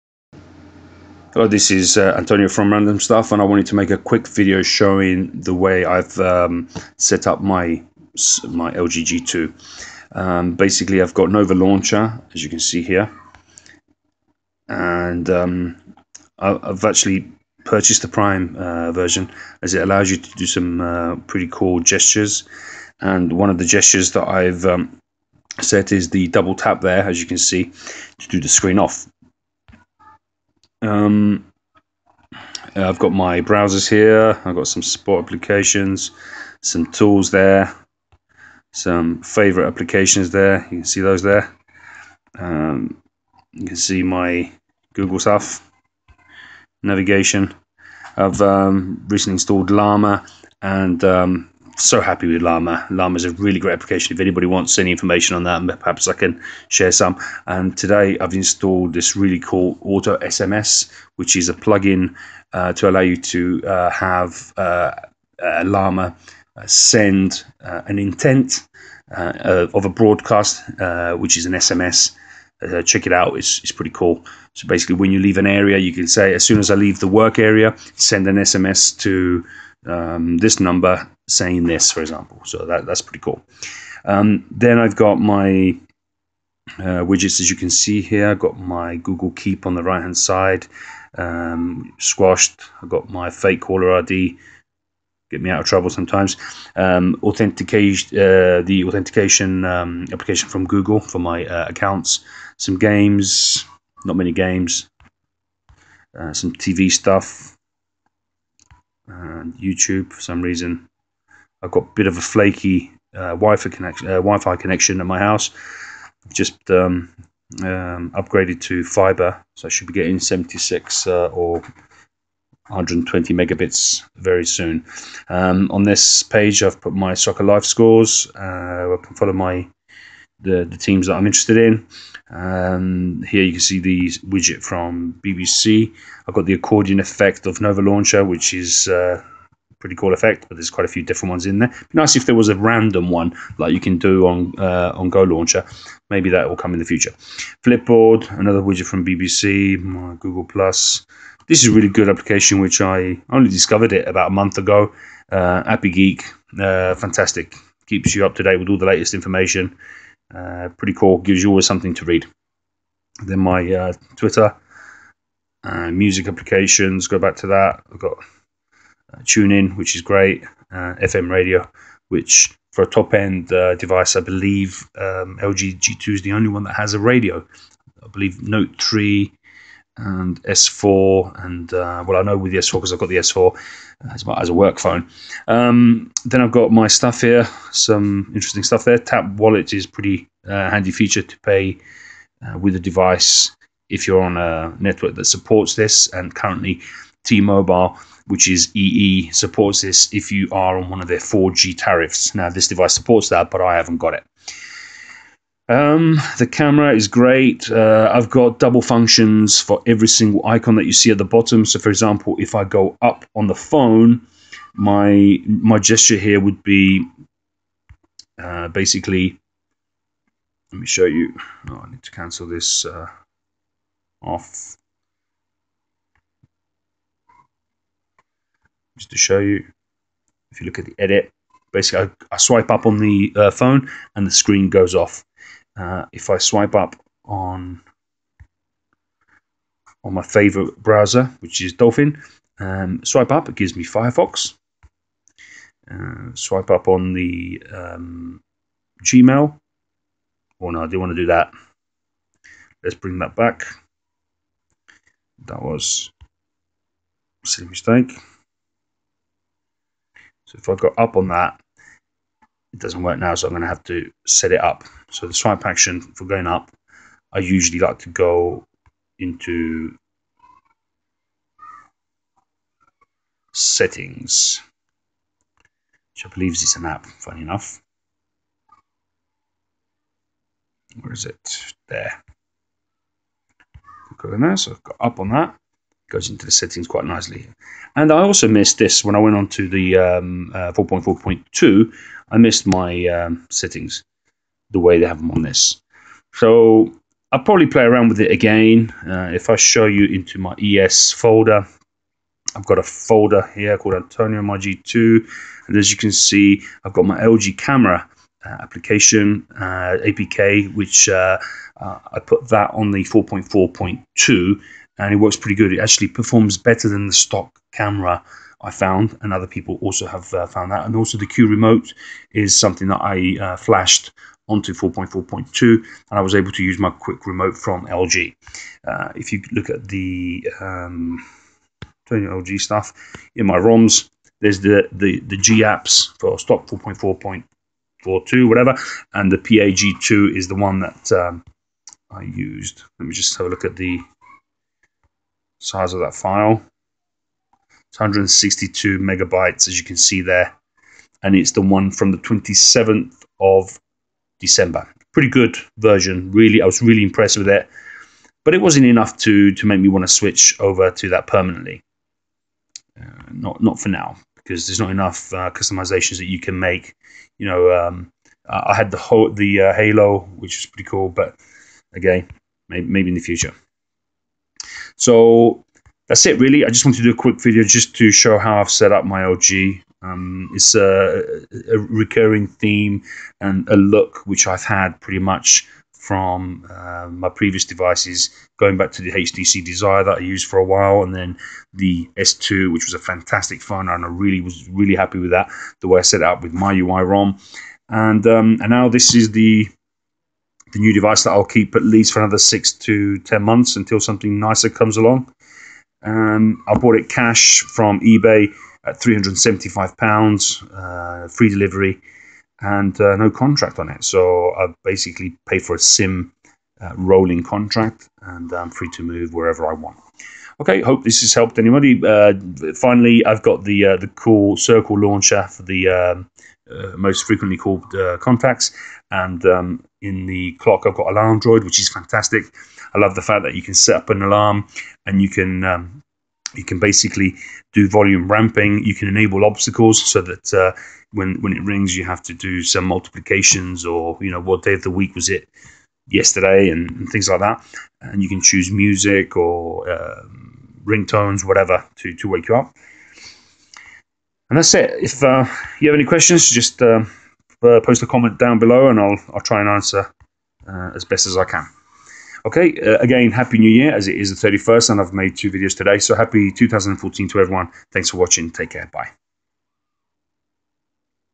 LG G2 + Nova Launcher - my setup and screens on stock LG 4.2.2, Llama, auto SMS text messages
Another quick video on my LG G2 to show how I set it up...that's all for this one.

"links that you may find interesting"